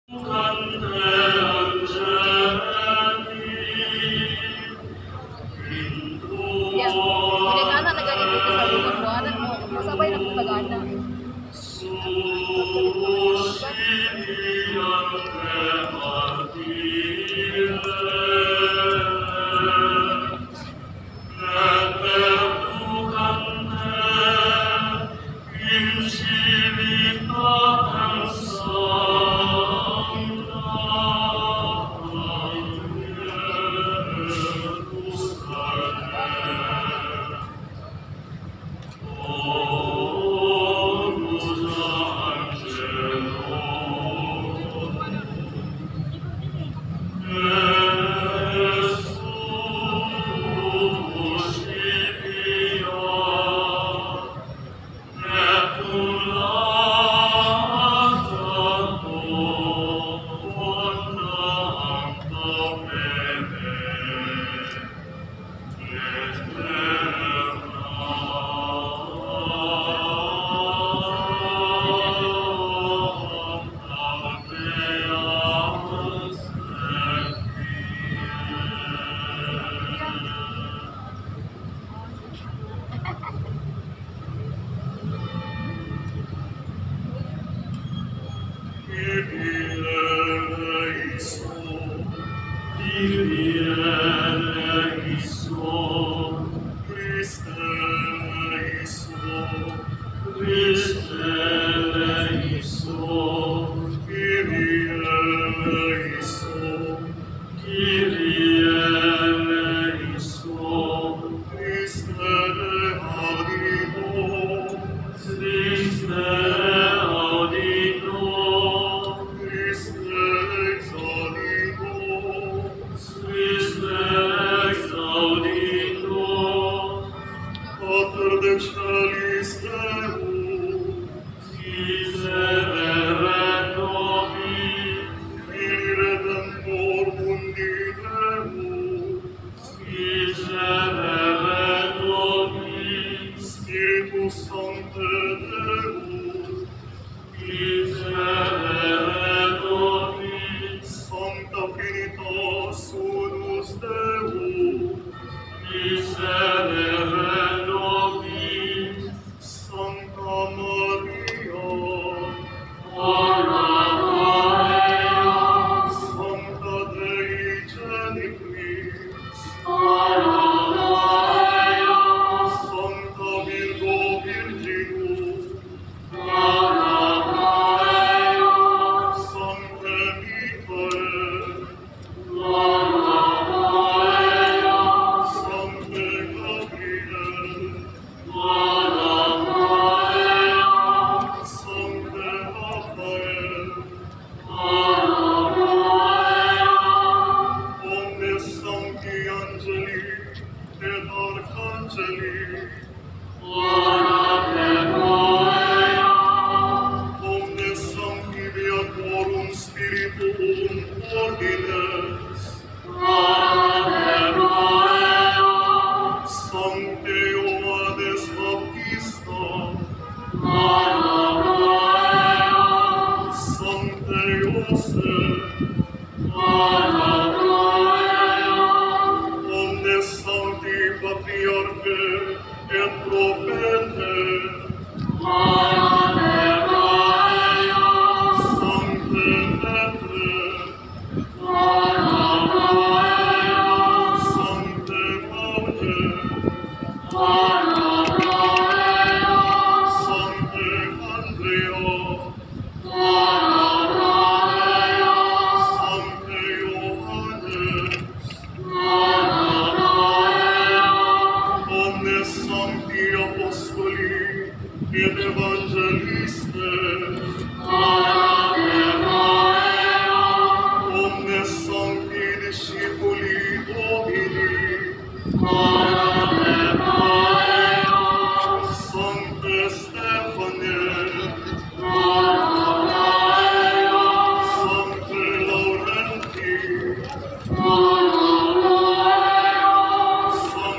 Funeral Mass for His ExcellencyMost Rev. Fernando R. Capalla, D.D.Archbishop - Emeritus of Davao
Christian Funeral Mass
for
His Excellency
Most Rev. Fernando R. Capalla, D.D.